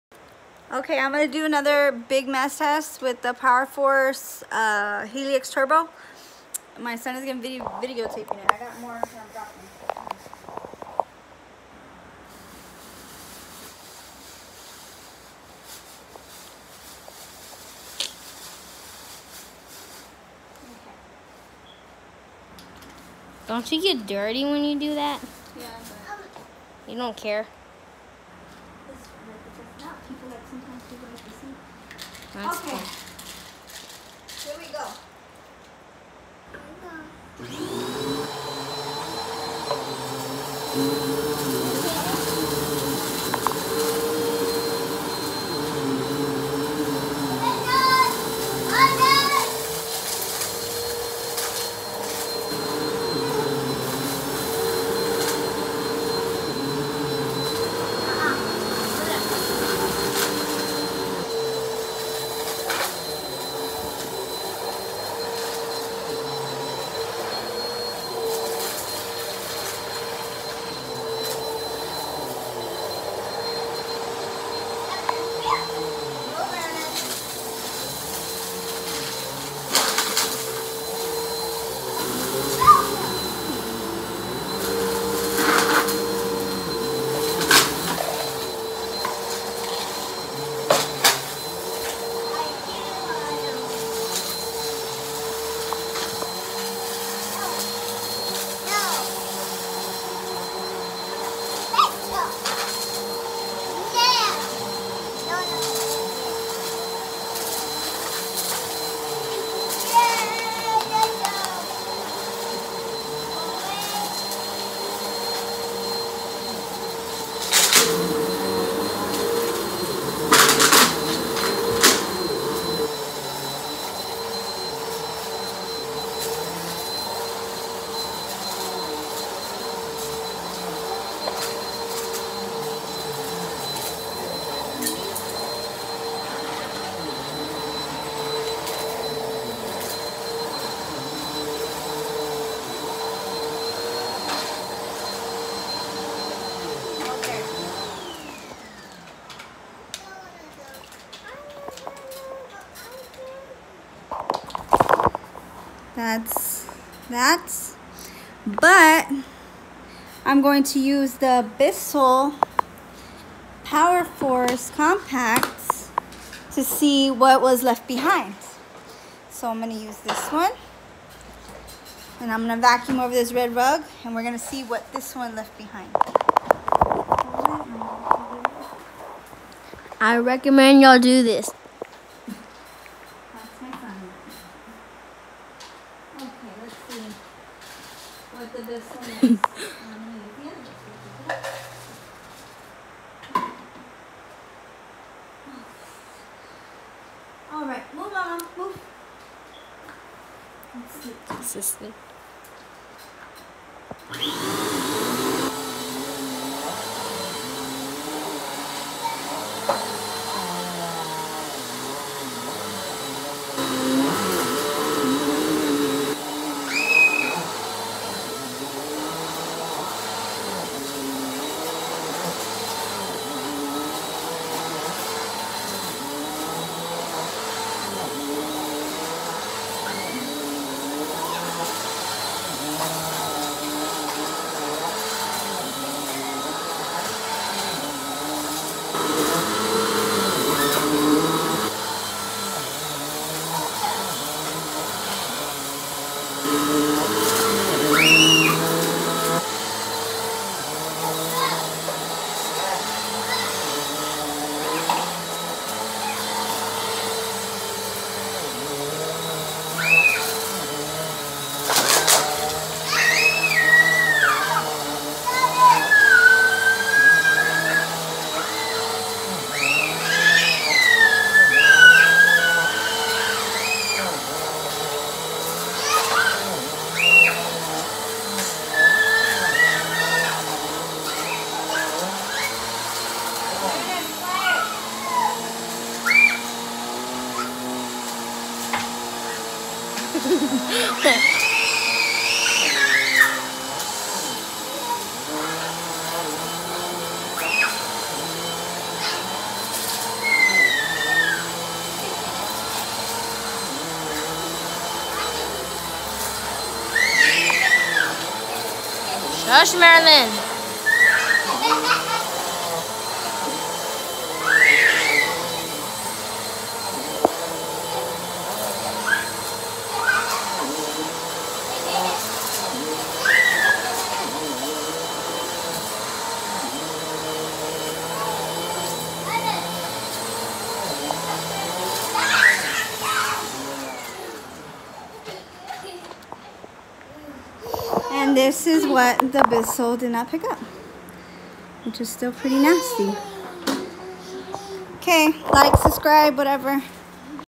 Bissell fun ( Big mess test) Bag Dump
Bissell power force compact picks up what bissell power force helix turbo didn’t.